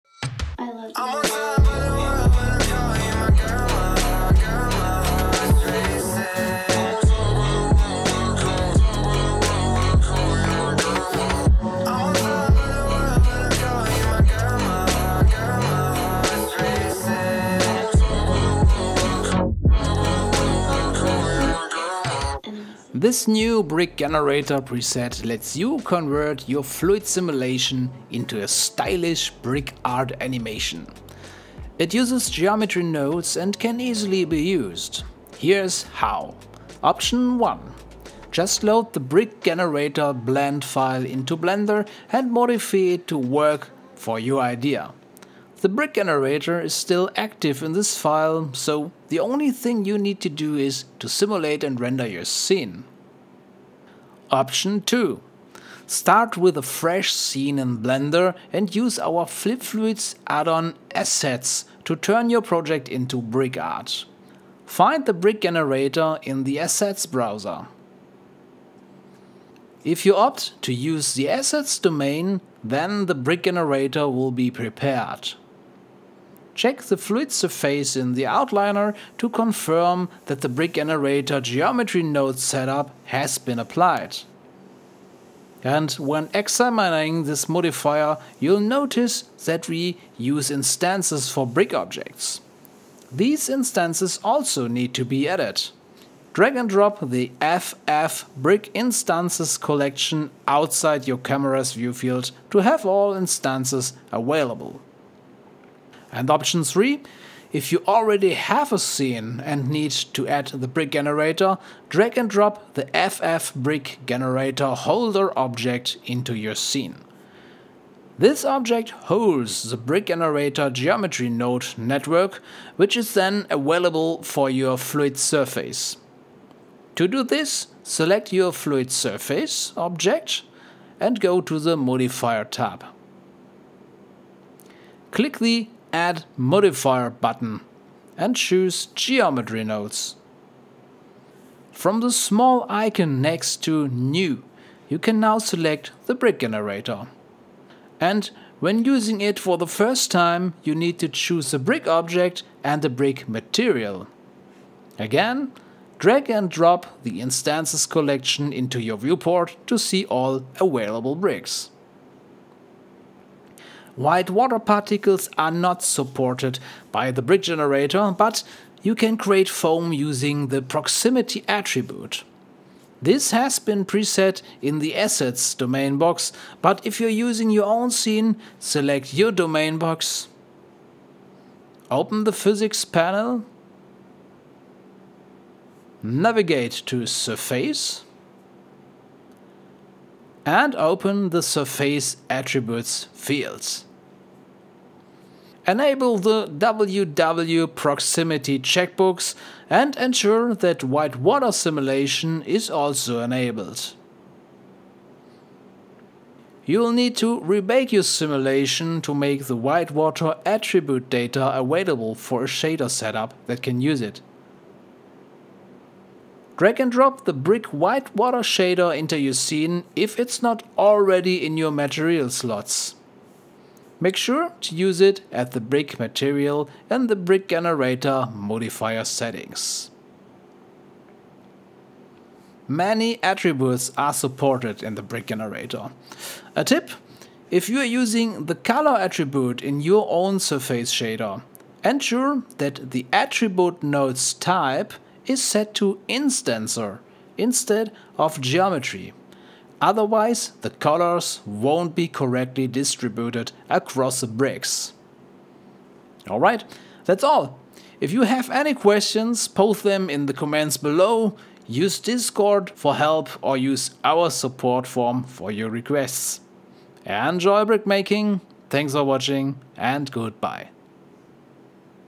Brick Generator for FLIP Fluids Addon 💧🔥
🌊 Introducing the Brick Generator: Dive into a new realm of liquid simulation with our latest Geometry-Nodes based preset for the FLIP Fluids Addon. Transform ordinary fluid surfaces into stunning brick-like structures, adding a unique touch to your motion designs.

🧱 Versatile and Dynamic: The Brick Generator seamlessly converts fluid surfaces into captivating brick patterns. Utilize our advanced attributes system to integrate additional effects such as foam or color mixing, elevating your creations to the next level.

💡 Application on Existing Simulations: Already have a finished simulation? No problem! The Brick Generator can be applied to pre-existing simulations. Note that it relies on already calculated attributes, so some resimulation might be necessary for full effect.

🎨 Endless Possibilities: The potential applications are virtually limitless. Whether it's for architectural visualization, abstract art, or realistic water simulations, the Brick Generator opens up a world of creative possibilities.

👀 We Can't Wait to See Your Creations: Our community's creativity knows no bounds, and we eagerly anticipate the inspiring and innovative animations you'll create with this new tool.